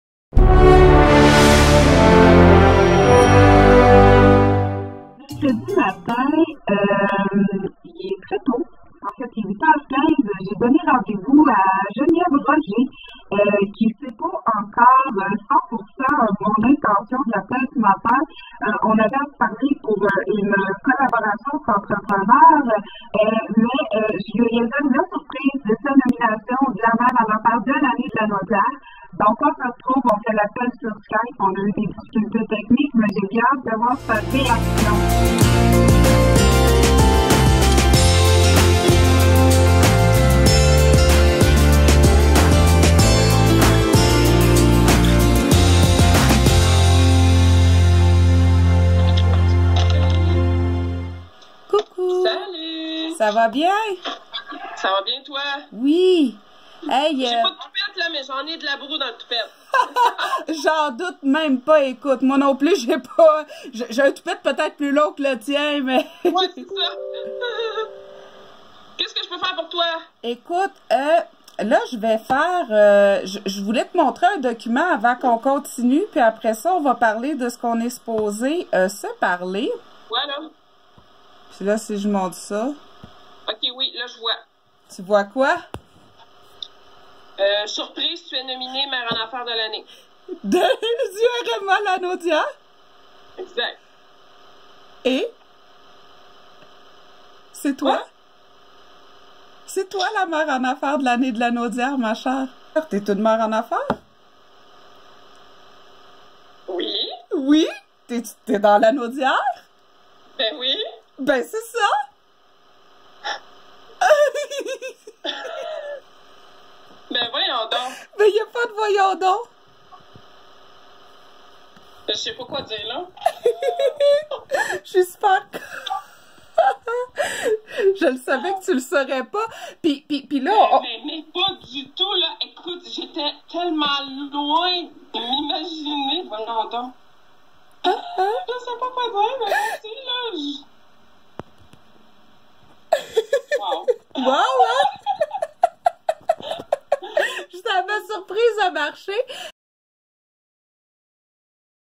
Geneviève Roger Mère en Affaires de l'Année Lanaudière
Félicitations Geneviève Roger de Média sans faute pour sa nomination!